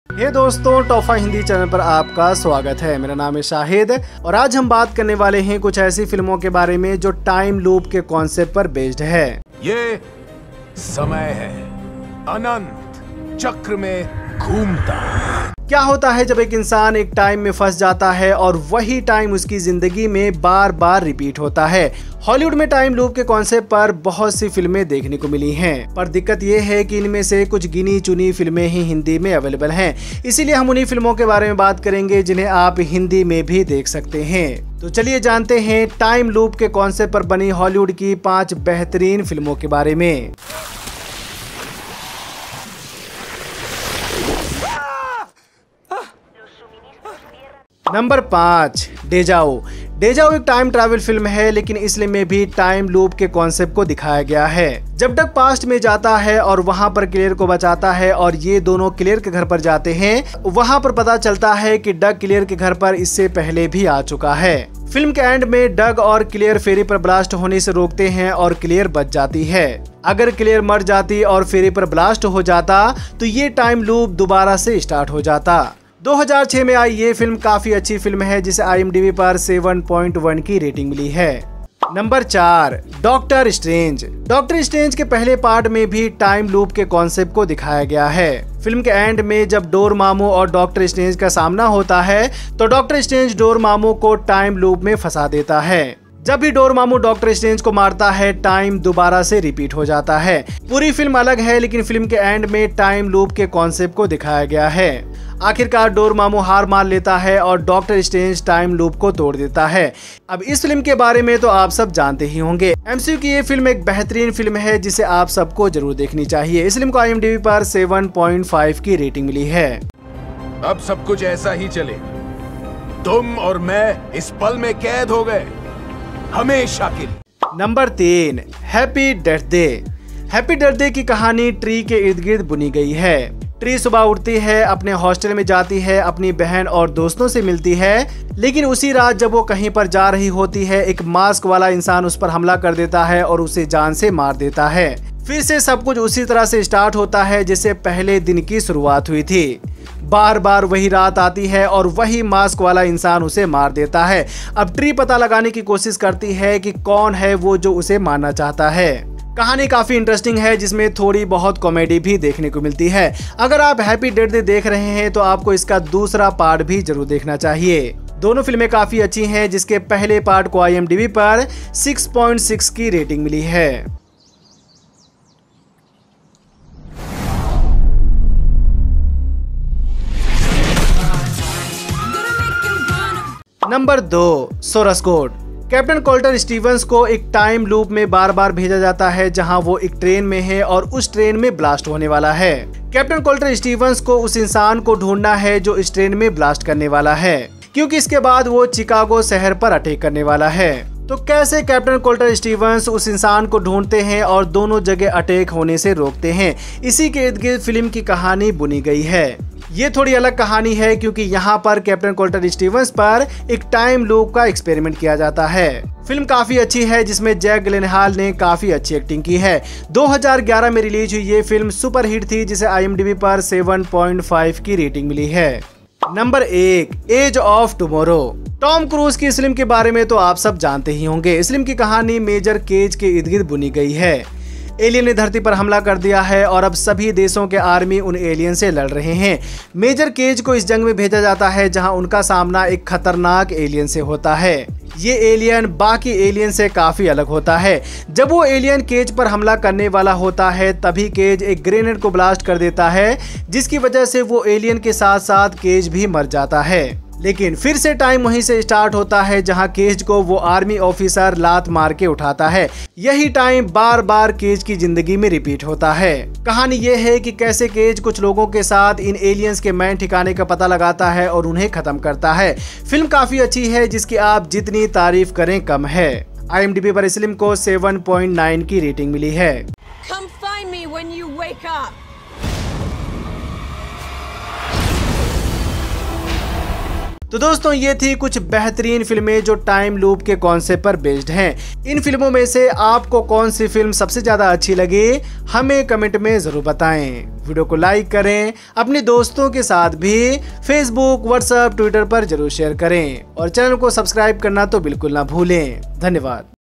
Top 5 Best Hollywood Time Loop Movies in Hindi
Hello Dosto Swagat Hai Aap Ka 'Top 5 Hindi' Channel Per:
Aaj Hum Baat Karne Wale Hain time loop par bani behtareen films ke bare mein, jo hindi me hain...